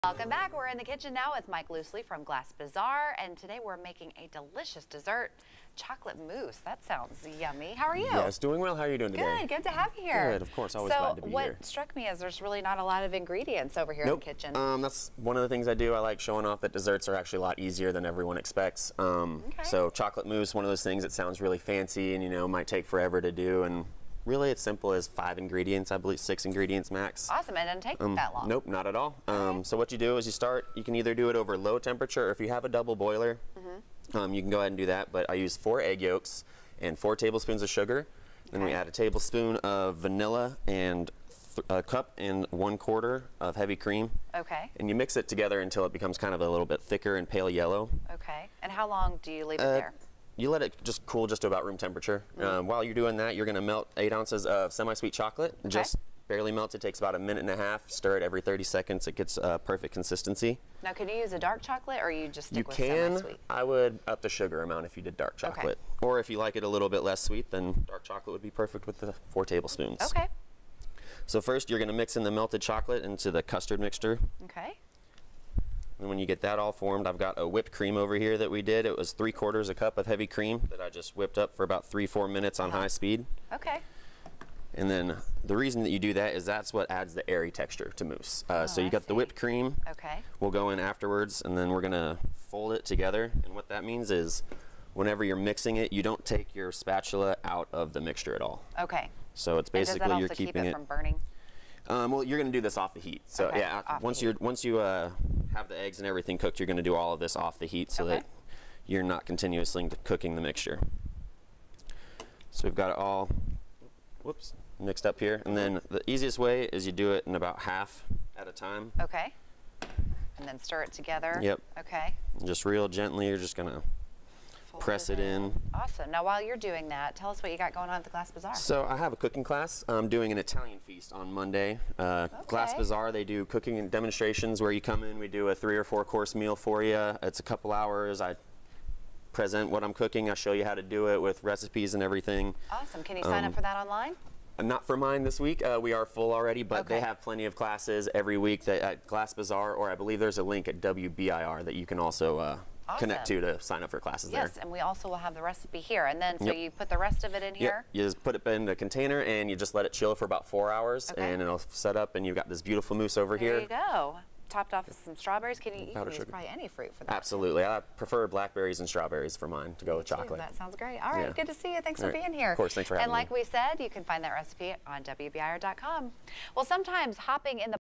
How to make chocolate mousse
we are in the kitchen with Mike Loosli from Glass Baazar and today we are making a delicious dessert: chocolate mousse.